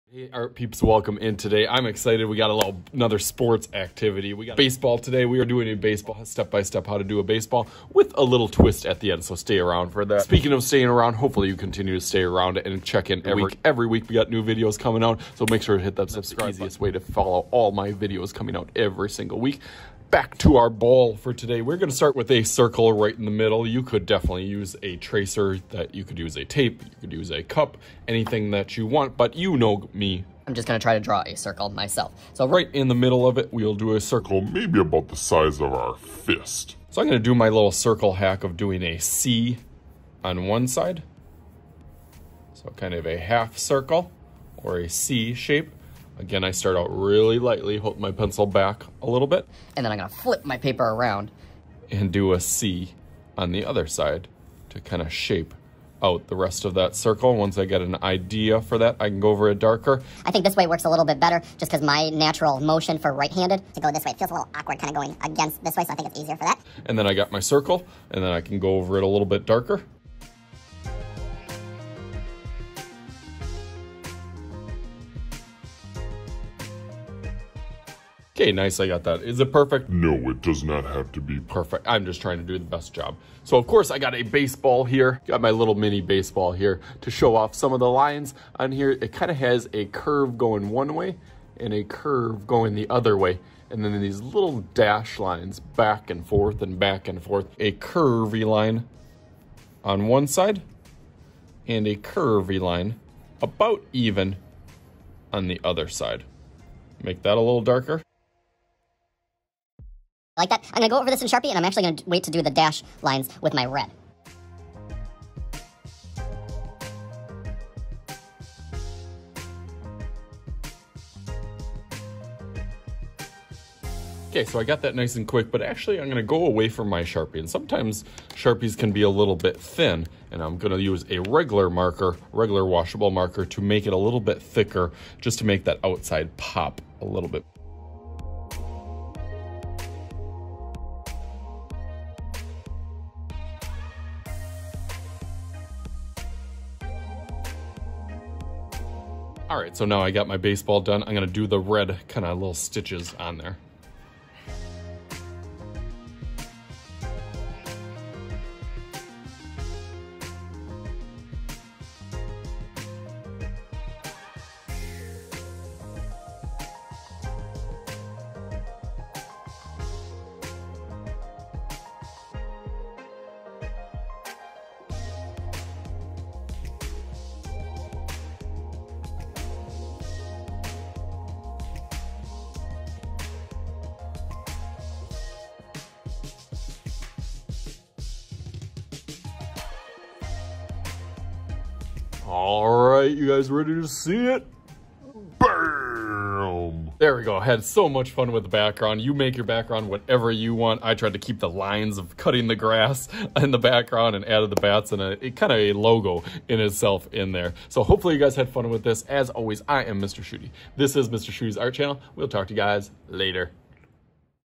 How to Draw a Baseball EASY - with Background for kids
This week let's Draw a Baseball ! This step by step is easy and fun for Any Artist to do- Grab ur Tools & Follow Along!

!New!

Join us w/ DAILY Updates on Both - - -